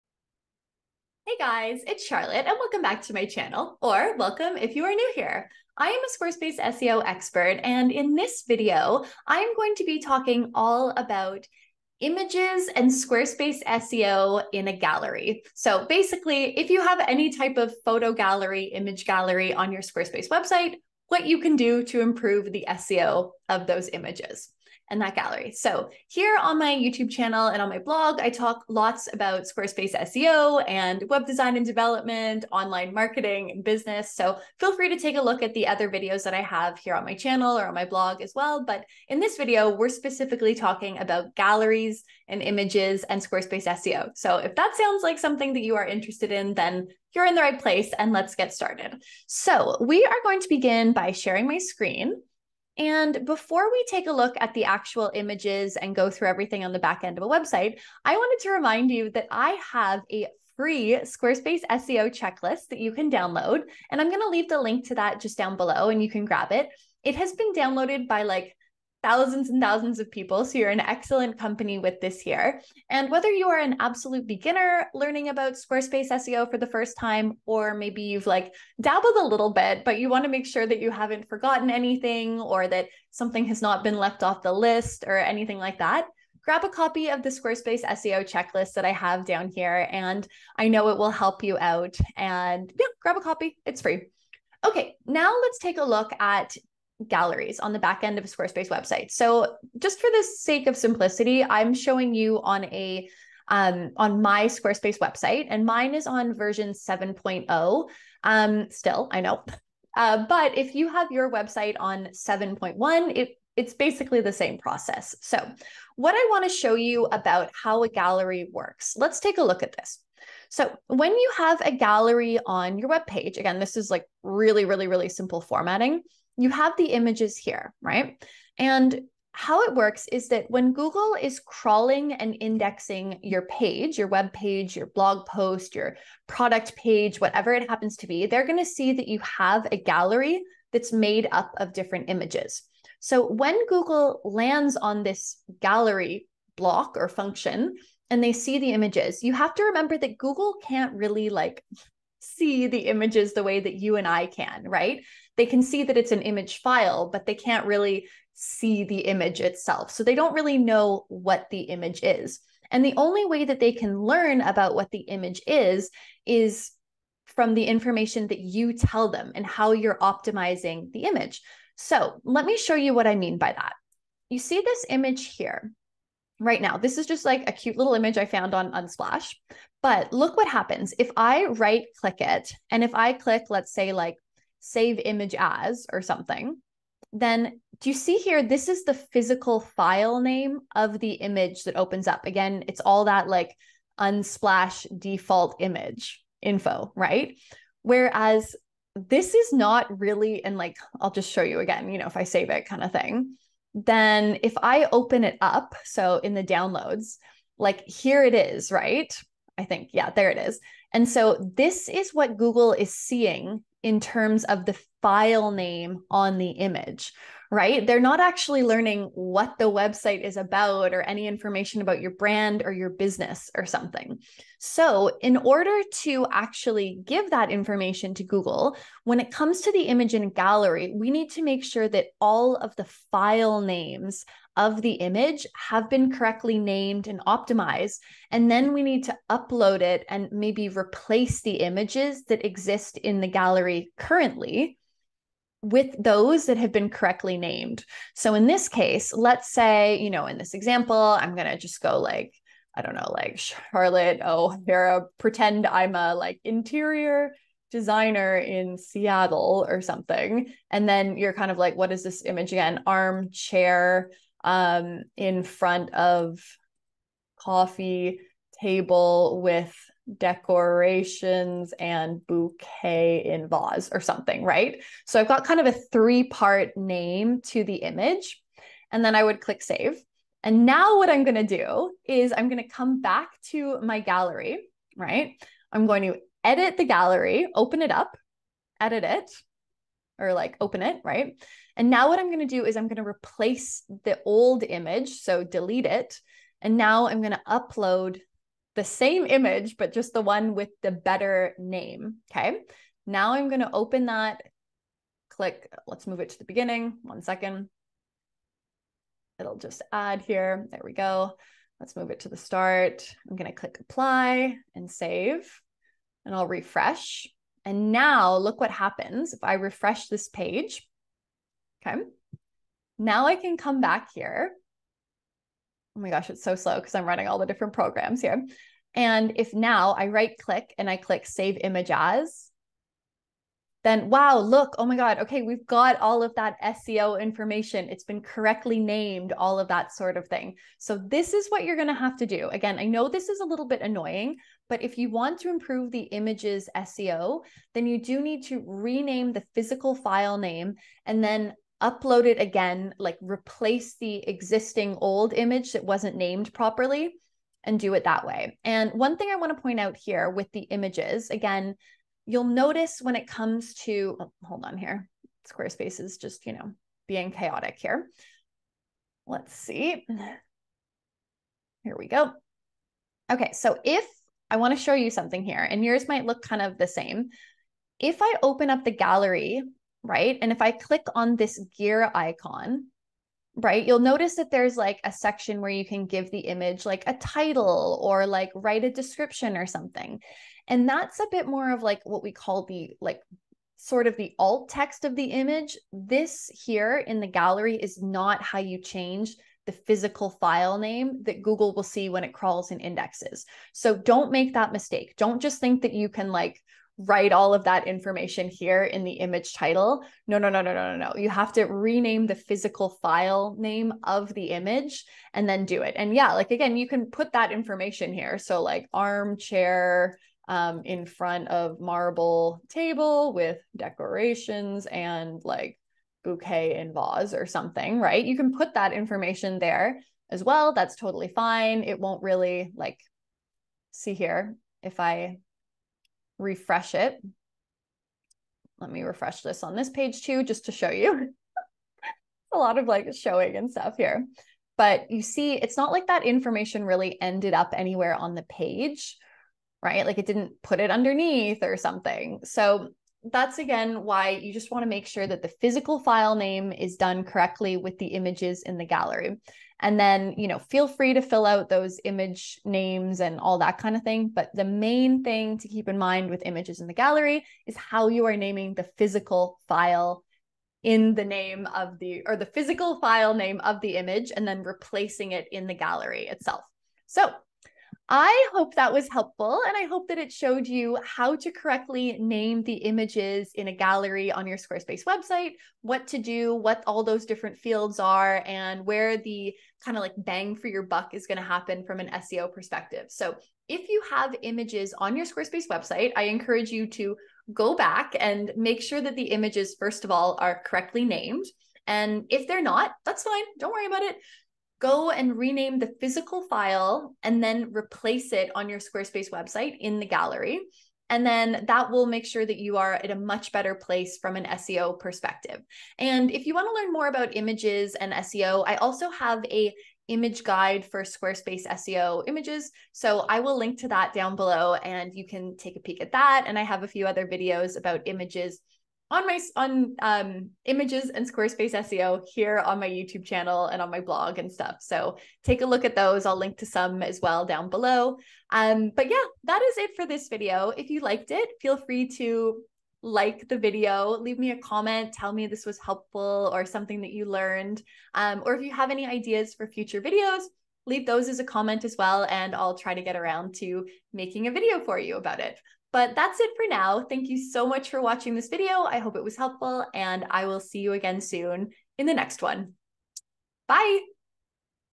Images & Photos in a Gallery and Squarespace SEO
IMAGES & PHOTOS IN A GALLERY AND SQUARESPACE SEO (read the blog post):

Hello SEO's and welcome to today's video all about images & photos in a gallery and how they affect SEO on your Squarespace website!

If you are using a Squarespace GALLERY block on a page/blog post/product page on your website, you will notice that they work a little differently than regular Squarespace IMAGE blocks. As such, there are different SEO considerations to make sure you are getting the most out of it and setting things up properly!

The biggest thing is making sure you are naming the image's physical file name correctly before including it in the gallery but that's not everything so be sure to watch the full video to learn more!

Time Stamps:
0:00 Intro
0:50 Squarespace SEO Checklist
1:36 Photos in Galleries & Google
3:50 Image file - rename & replace for better SEO
6:18 Image title & descriptions in the Gallery
7:59 Outro & final thoughts

Sit back, relax, open your own Squarespace website to follow along with me, and hit play whenever you're ready! I'm so excited to teach you more about the wonderful world of Squarespace SEO!

-Charlotte